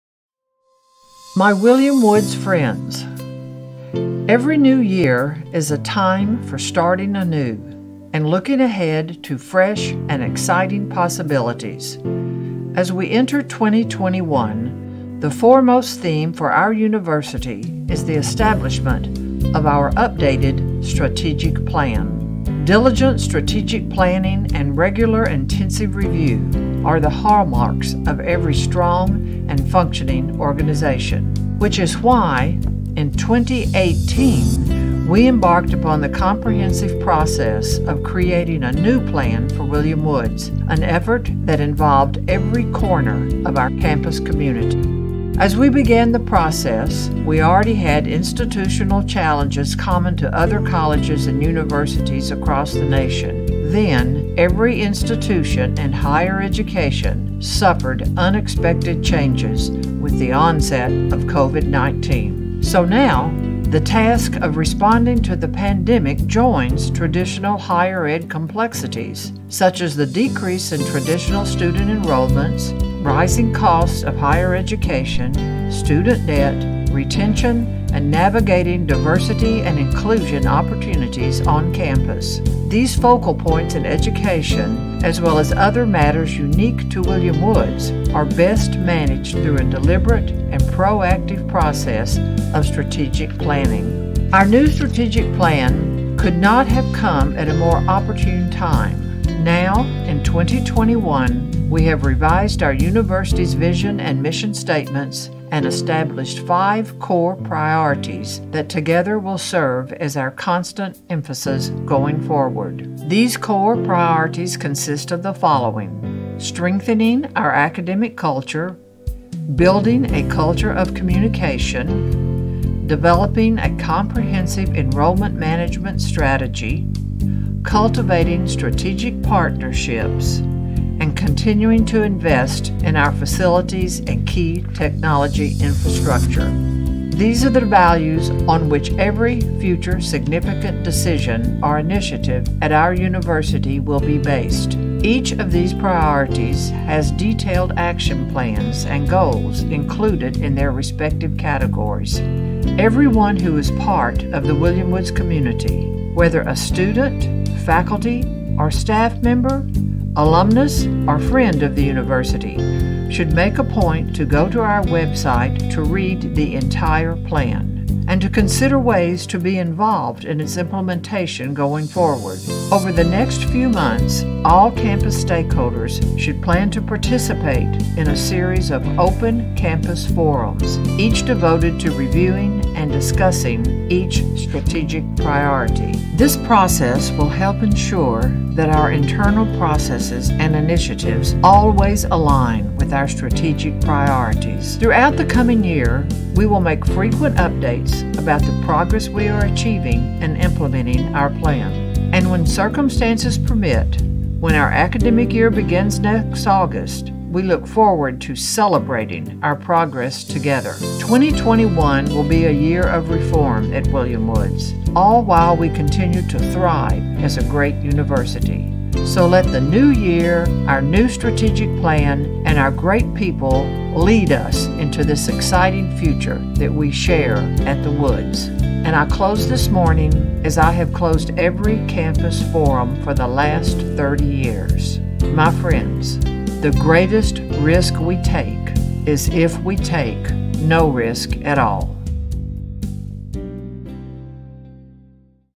William Woods University Strategic Plan
An independent voice in higher education, William Woods University is pleased to share the Strategic Plan as introduced in the spring of 2021. WWU distinguishes itself as a student-centered and professions-oriented university committed to the values of ethics, self-liberation, and lifelong education of students in the world community.